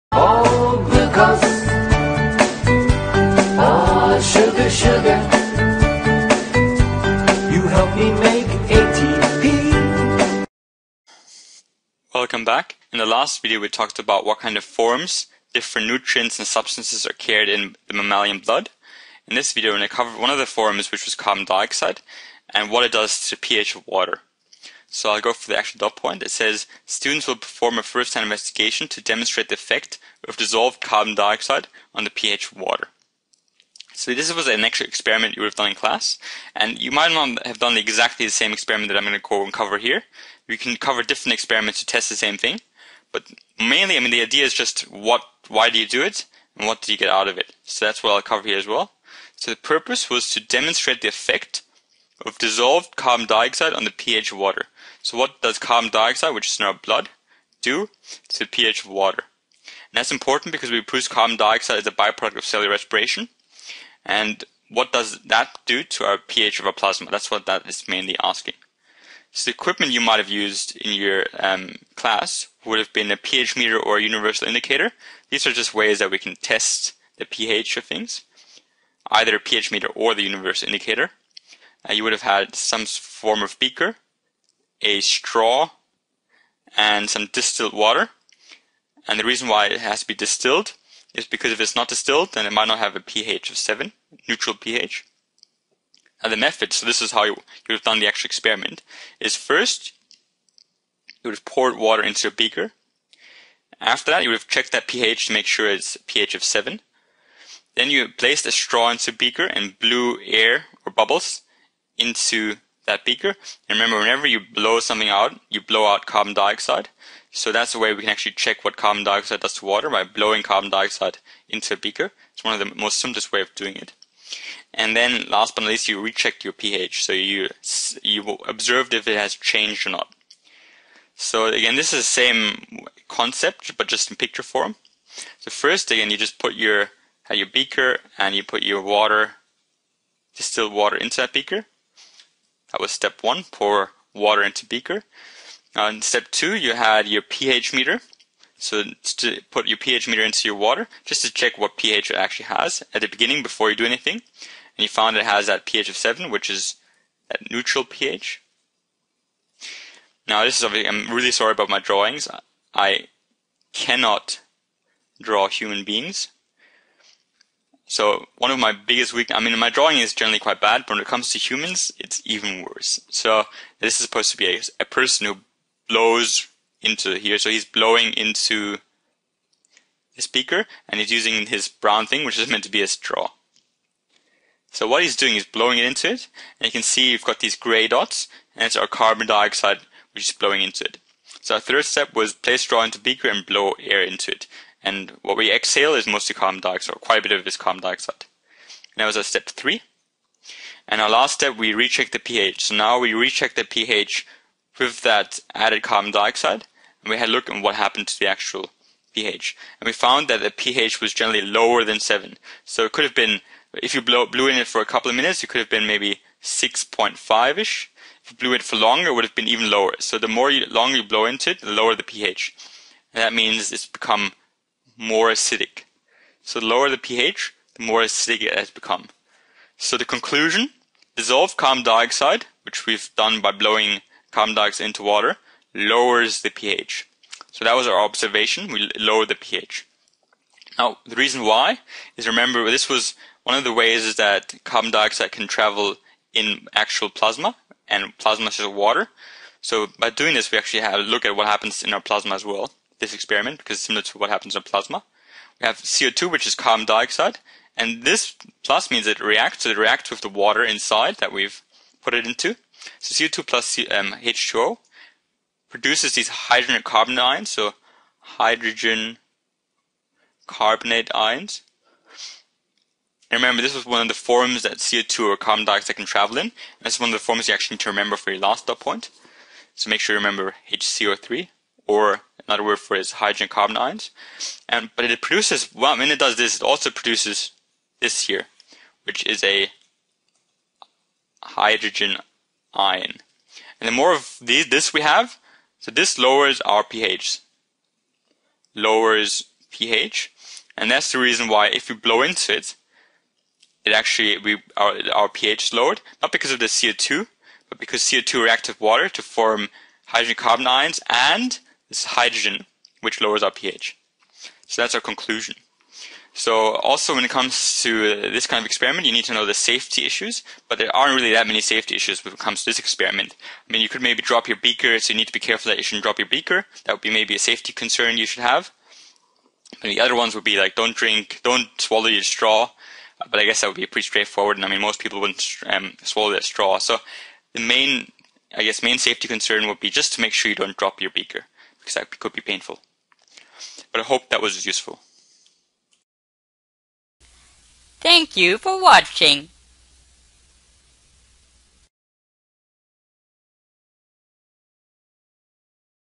MB.2.2. Carbon-dioxide and pH experiment (HSC biology)
Covers the HSC biology syllabus dot point: "perform a first-hand investigation to demonstrate the effect of dissolved carbon dioxide on the pH of water"